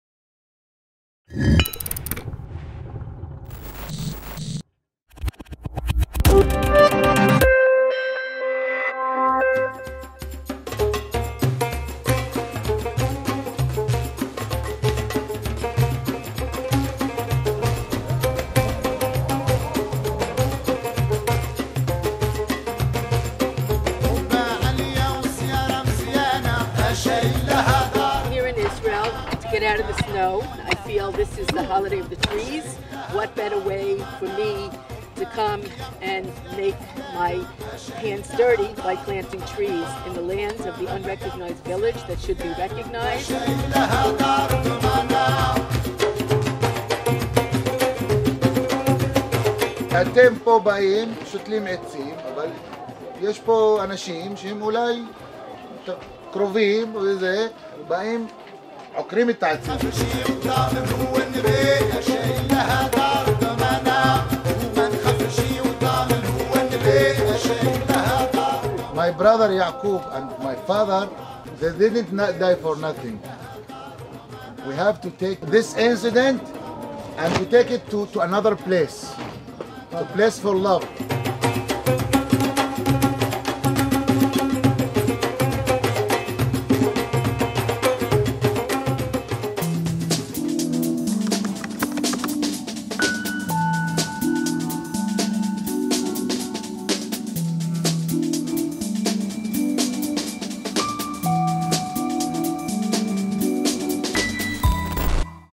Tu Bishvat in Umm-al Hiran
What a better place to celebrate Tu Bishvat than in Umm-al Hiran? After the demolitions last January 18, Umm al-Hiran is still under the threat of being completly destroyed.